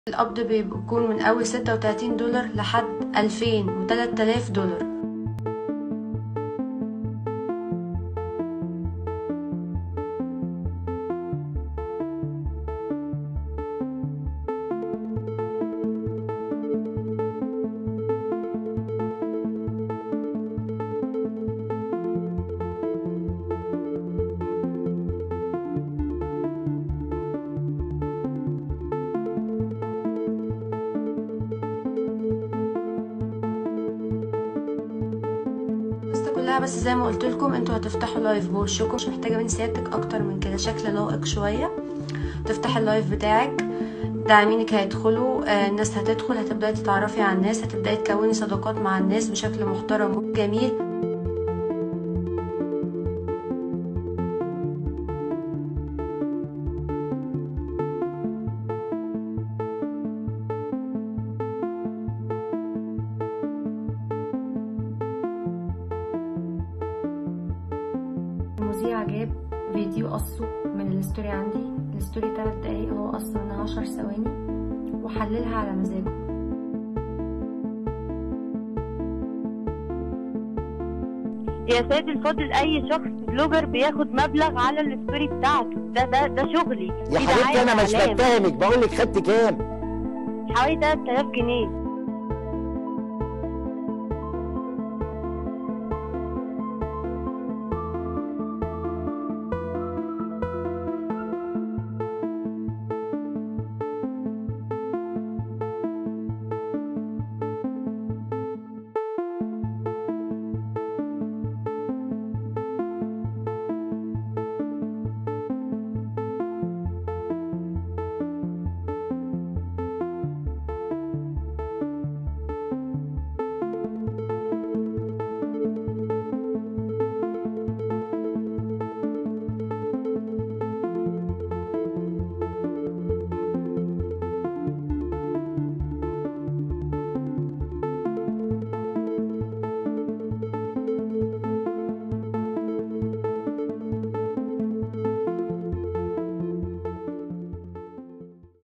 فخ الأموال السهلة والأحكام المسبقة
التعامل مع قضية حنين حسام انقسم لهجوم ودفاع، من غير معلومات كافية عن الموضوع نفسه.. هنا جمعنا لكم المعلومات المتاحة عن القضية والتطبيق الذي تروج له سبب المشكلة